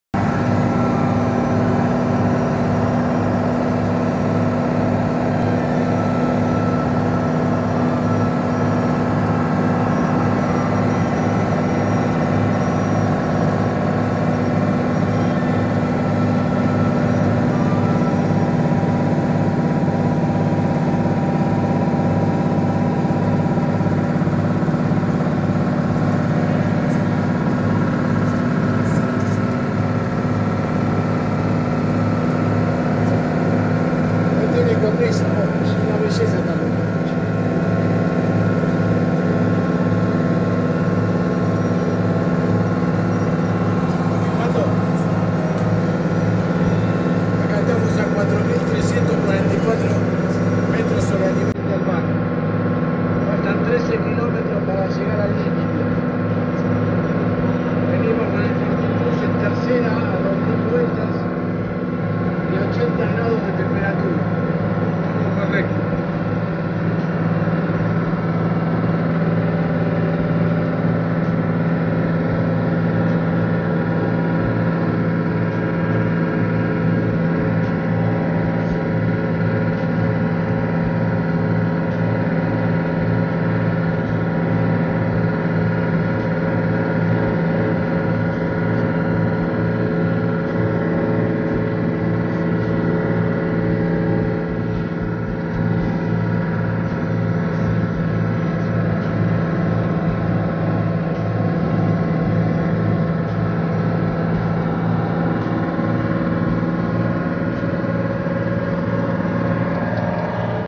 PASO SAN FRANCISCO, RN60 , CATAMARCA, ARGENTINA - LLEGANDO A PASO CON EL FORTINBUS
LLEGANDO A PASO SAN FRANCISCO CON EL FORTINBUS
A 4344 MSNM Y 13 KM PARA LLEGAR A LA FRONTERA
EN 3º VELOCIDAD Y 80ºC DE TEMPERATURA.
QUE MAQUINA!
El paso de San Francisco es un importante collado o paso montañoso en la cordillera de los Andes. Conecta los sectores centronorte de la República Argentina y República de Chile. El punto más elevado de este paso fronterizo se halla a una altitud de 4776 msnm

Podes seguir la historia del FORTINBUS, desde su adquisición hasta la fecha